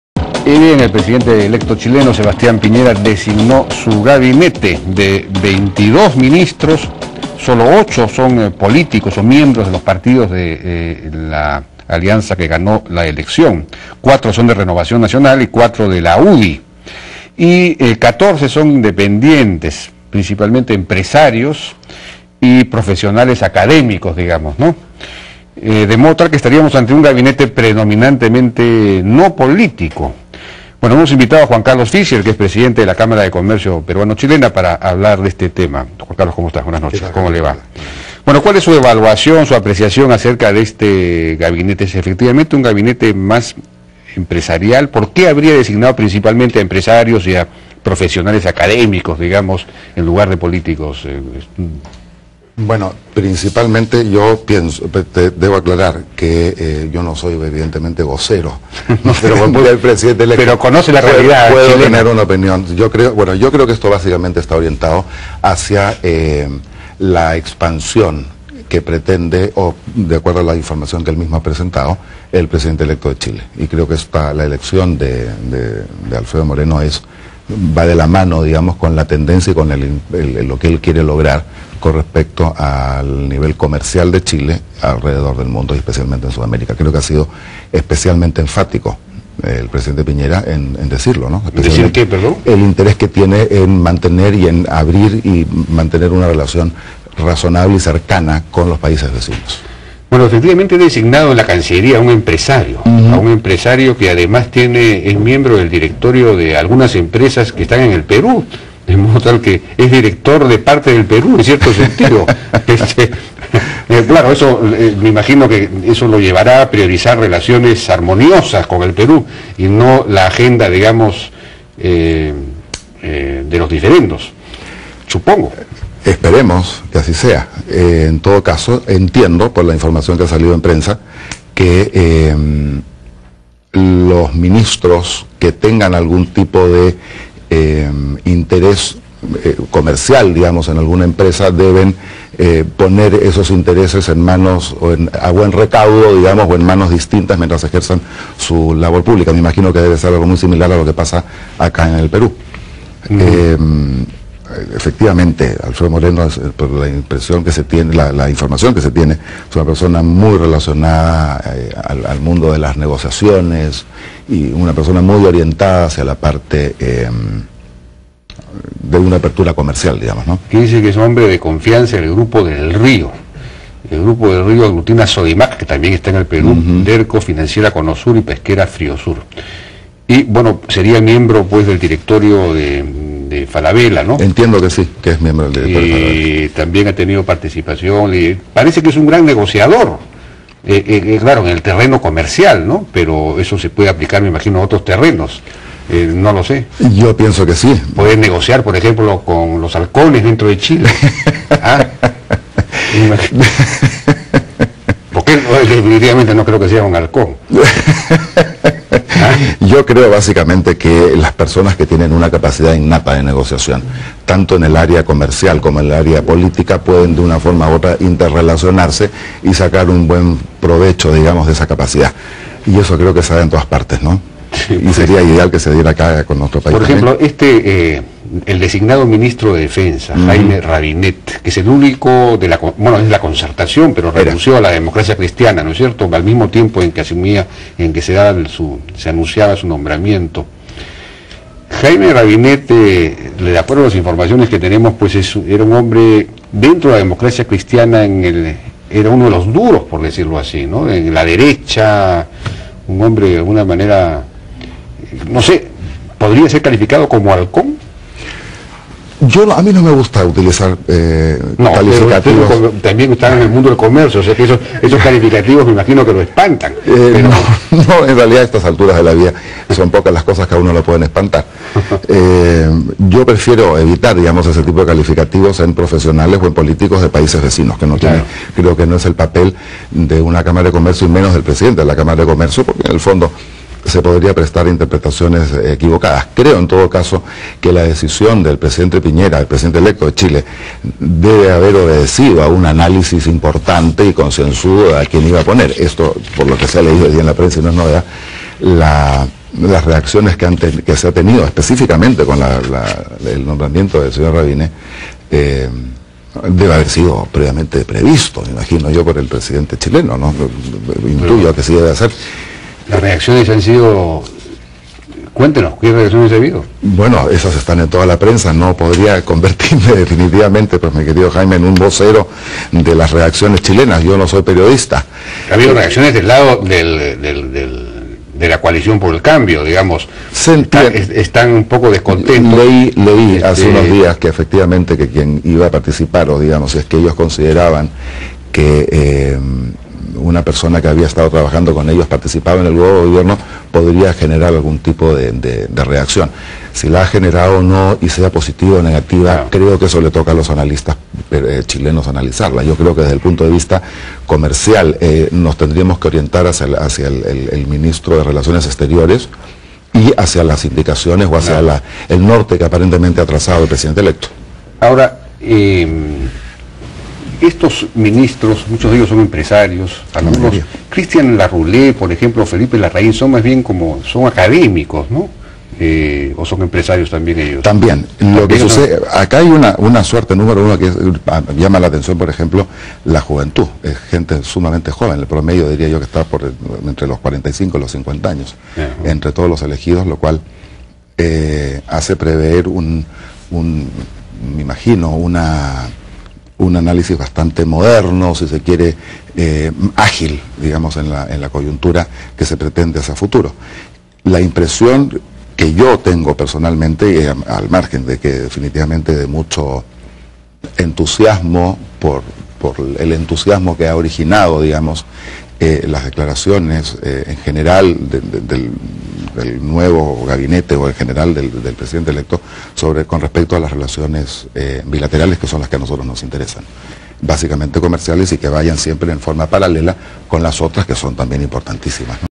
JUAN CARLOS FISHER, 1ra parte
JUAN CARLOS FISHER (Presidente de la Cámara de Comercio Peruano-Chilena)
Tema: Gabinete de Sebastián Piñera y relaciones Perú-Chile